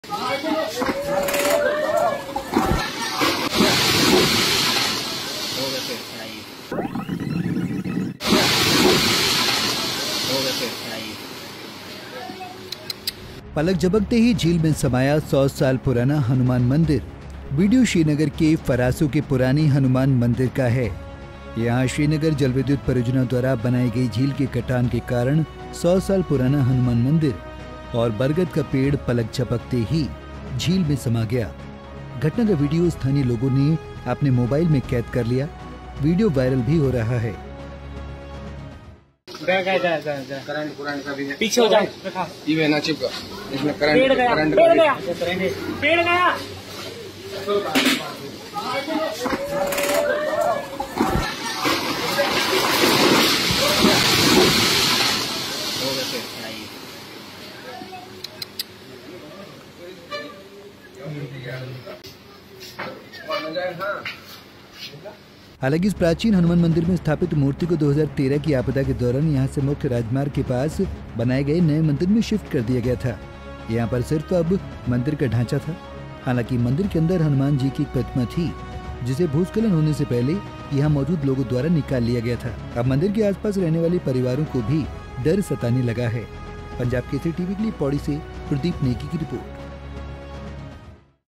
Uttarakhand heavy Rain: पलक झपकते ही झील में समाया 100 साल पुराना हनुमान मंदिर, देखिए वीडियो
पलक झपकते ही झील में समाया 100 साल पुराना हनुमान मंदिर